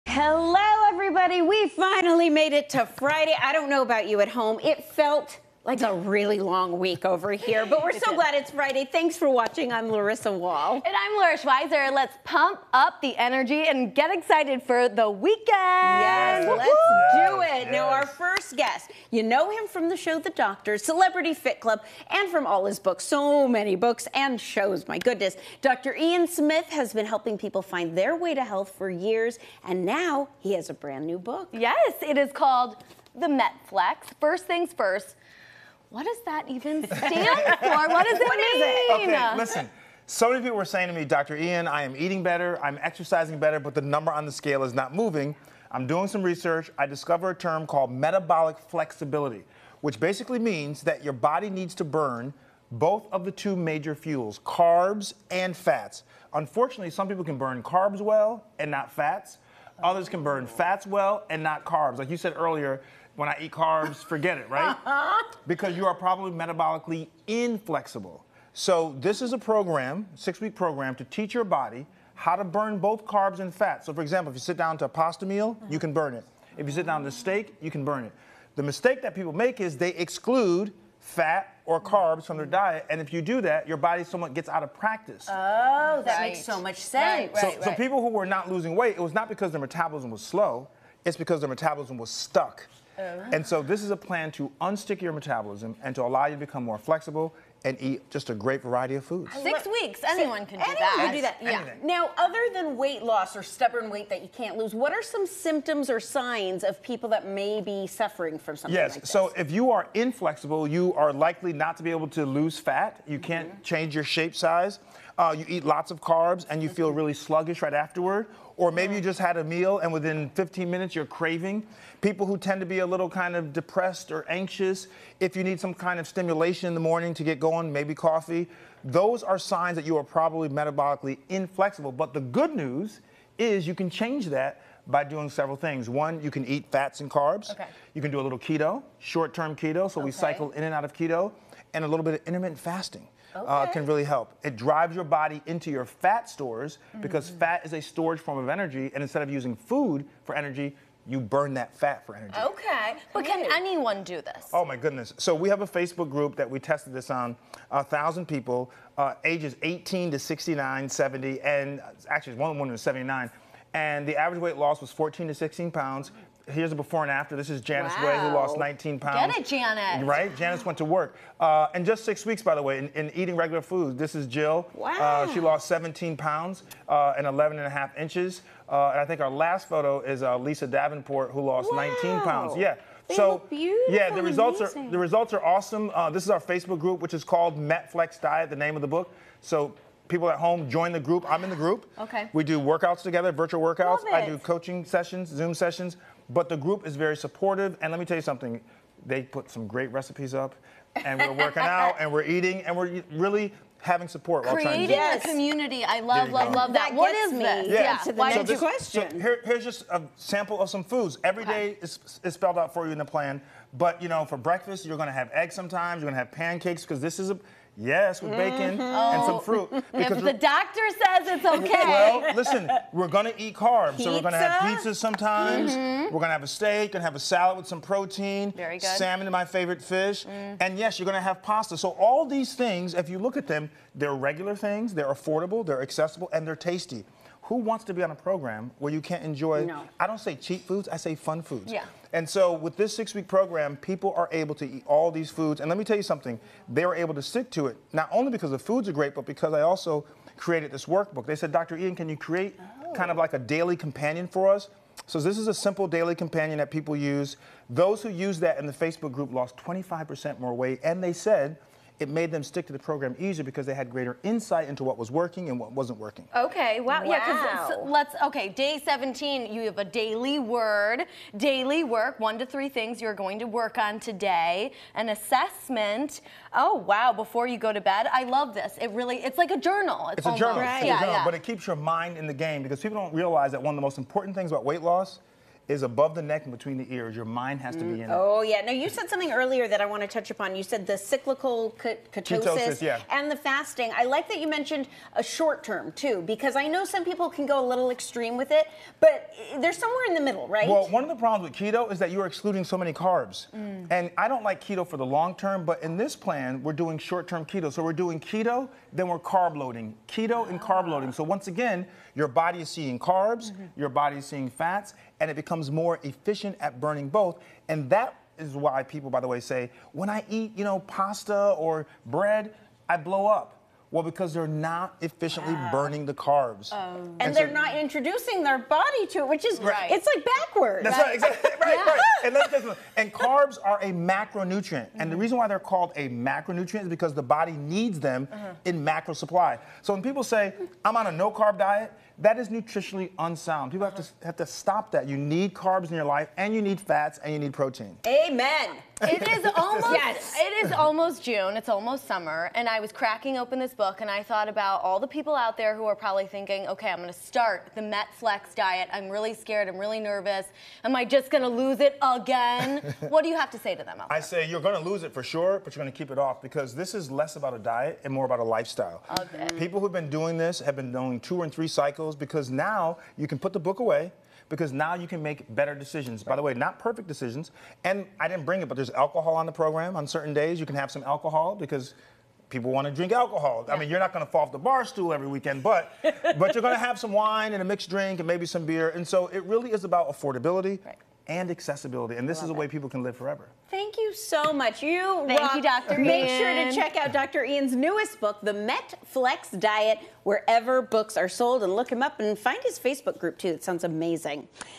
The real reason you're not losing weight according to Dr. Ian Smith
Purchase Dr. Ian Smith's new book "The Met Flex Diet" wherever books are sold.